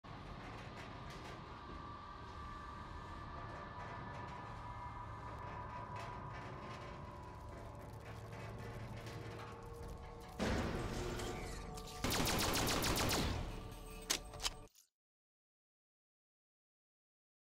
Elevator to Hell
ItachiWilt makes his way to the boss fight.